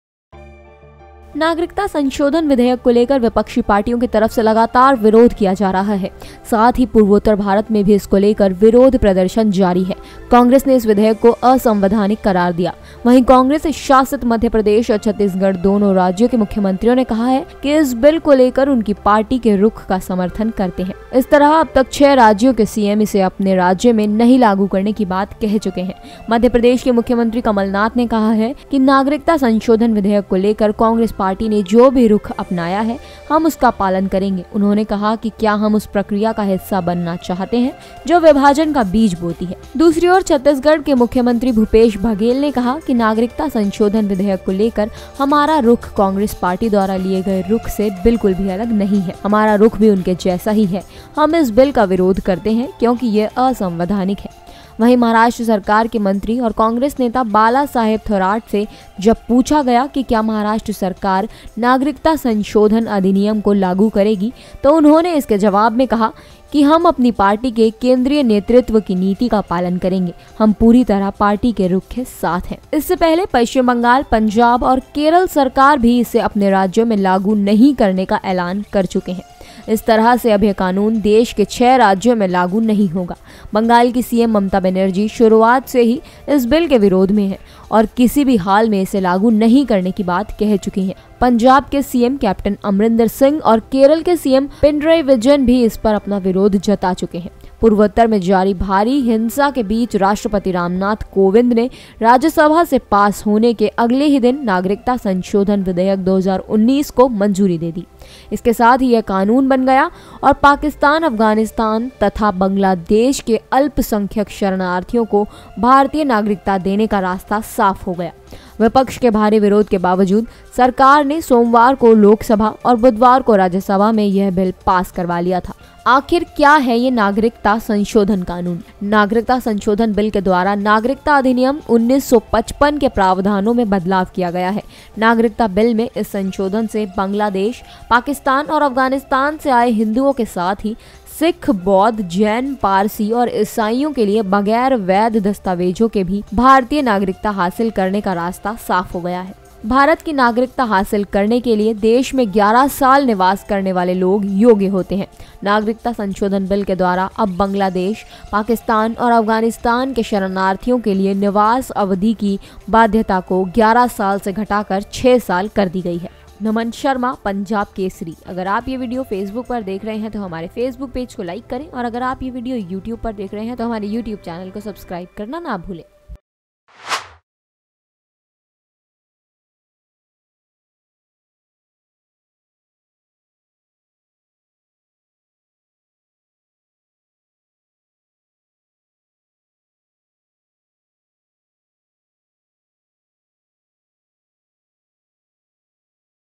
नागरिकता संशोधन कानून - राज्यों ने किया लागू करने से इनकार - Citizenship Amendment Bill implement
नागरिकता संशोधन कानून - राज्यों ने किया लागू करने से इनकार - Citizenship Amendment Act refuse to implement